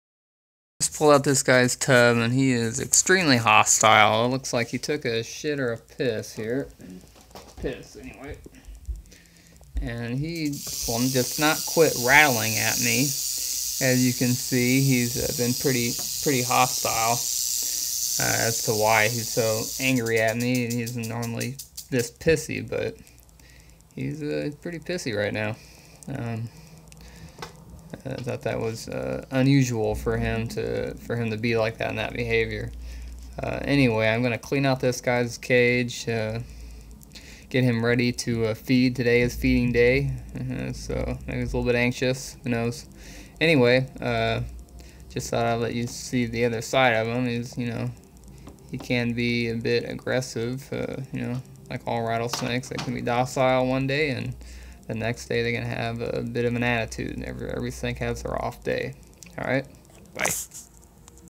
Cleaning time Not so happy
Crotalus oreganus doesn't seen so happy to see me, cleaning time..